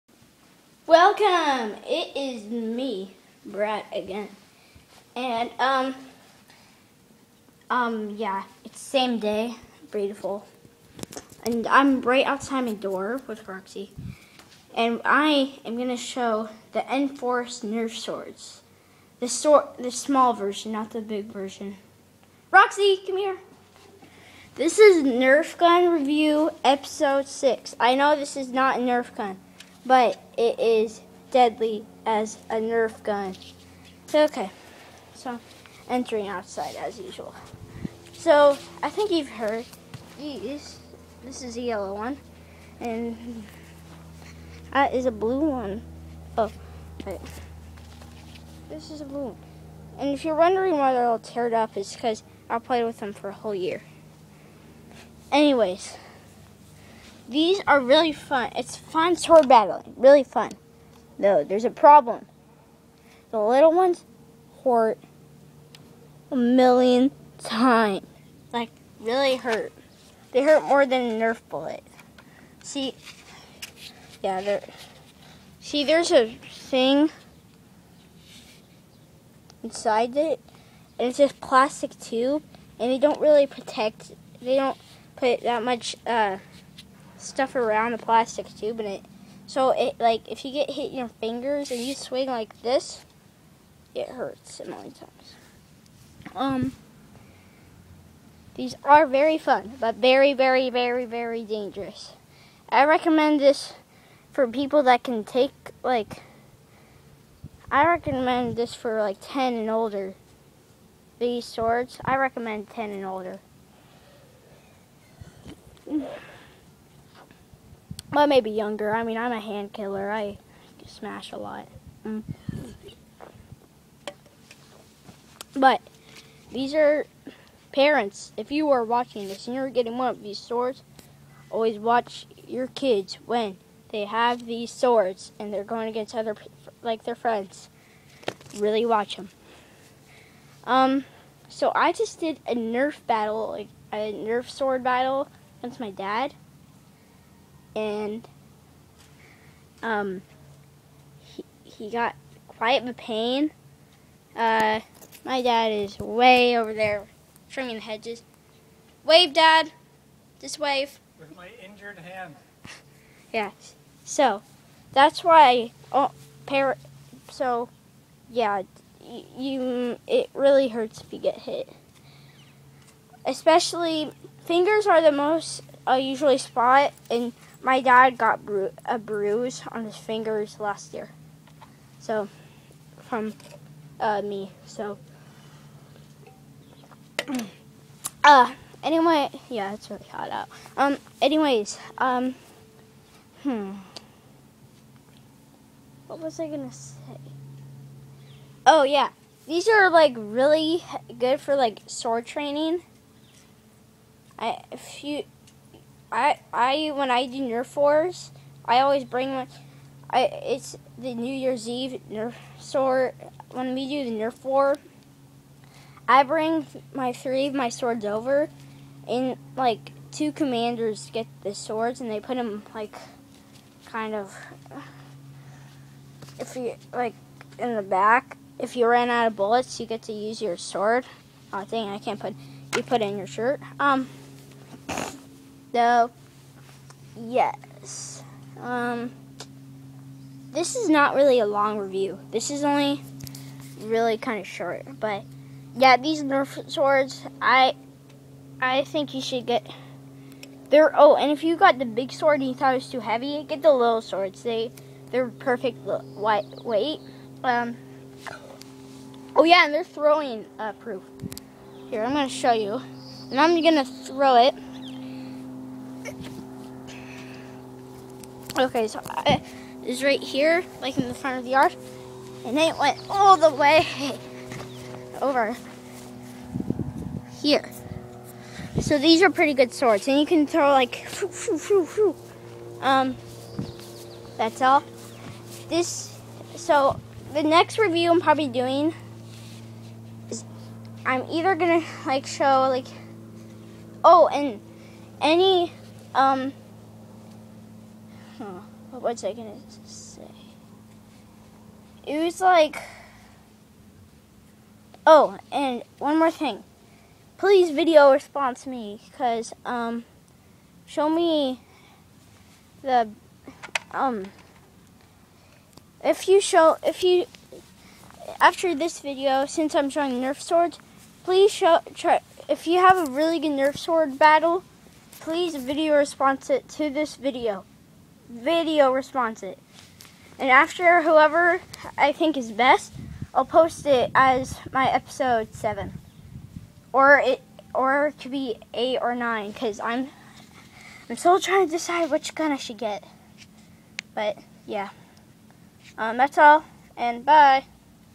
Nerf gun reviews episode 6
This is about the N-Force small nerf swords!
Please send a message or post a comment if you like this review please!